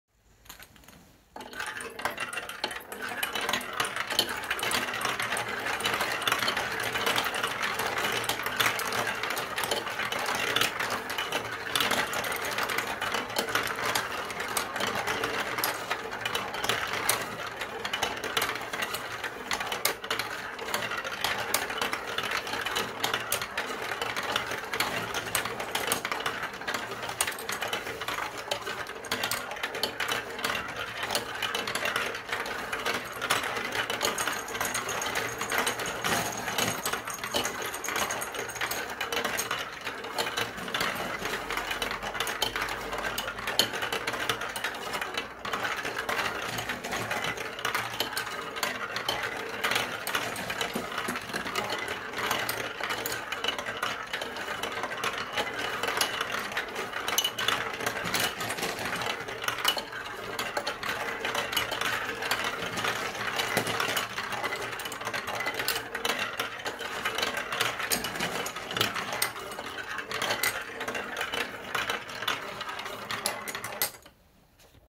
Bouncing Sounds Marble Run ASMR
The sound of rolling balls is soothing. Roll colorful balls and BB bullets and enjoy the difference in sound. # Marble Run # ASMR # Healing